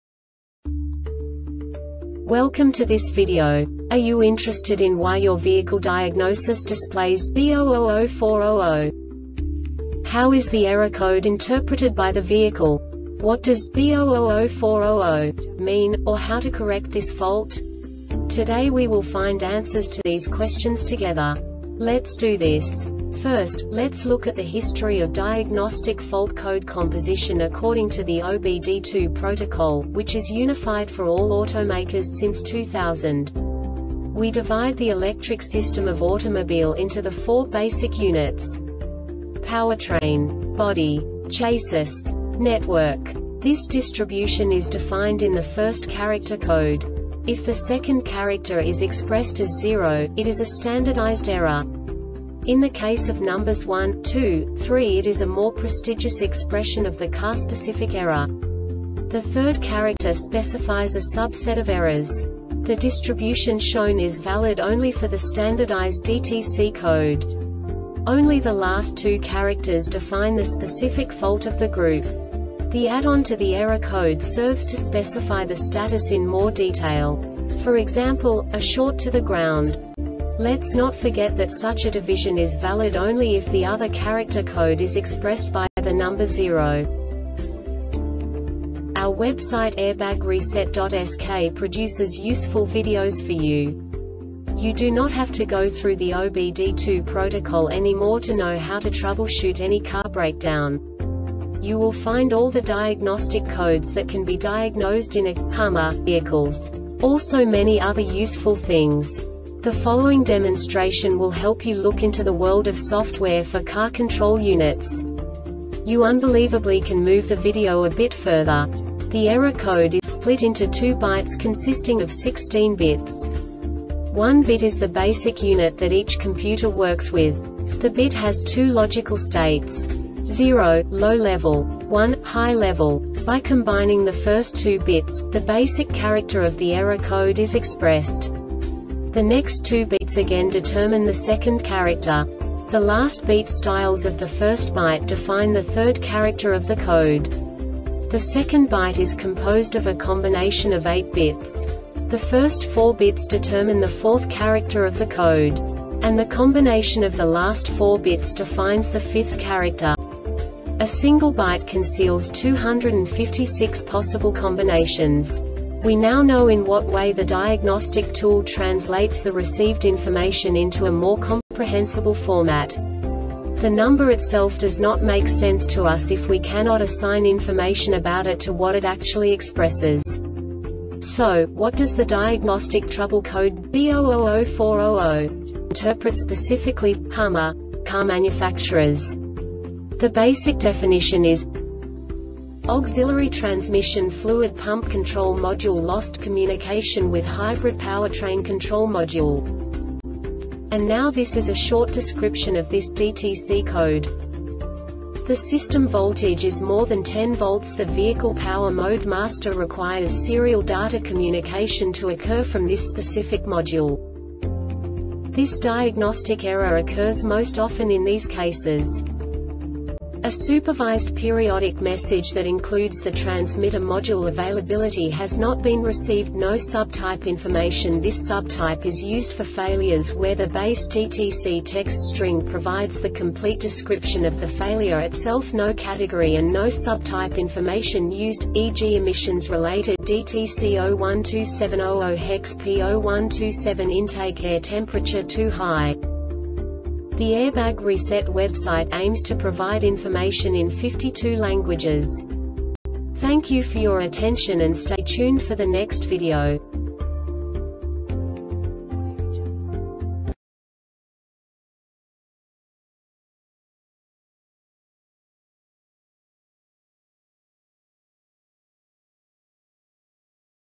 DTC Hummer B0004-00 Short Explanation
Contents:
0:21 Basic DTC analysis according to OBD2 protocol standard.
1:48 Insight into programming
2:58 A diagnostically understandable description of the Diagnostic Trouble Code
3:05 Basic error definition

Make:
HUMMER
Code:
B0004 00
Definition:
Auxiliary Transmission Fluid Pump Control Module Lost Communication with Hybrid Powertrain Control Module
Description:
The system voltage is more than 10 V.  The vehicle power mode master requires serial data communication to occur from this specific module.